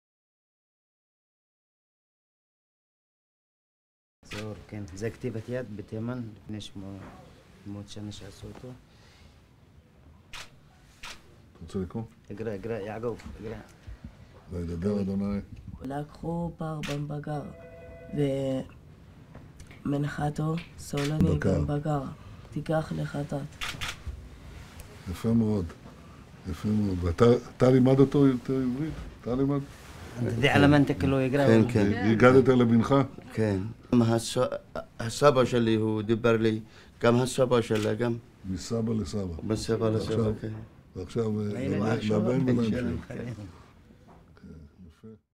Netanyahu con los judíos rescatados de Yemen en una operación secreta
Israel rescató a 19 Judíos de Yemen amenazados por la guerra en una “operación encubierta” para rescatar a algunos de los últimos restos de una de las más antiguas comunidades judías del mundo, dijeron las autoridades el lunes, según AFP .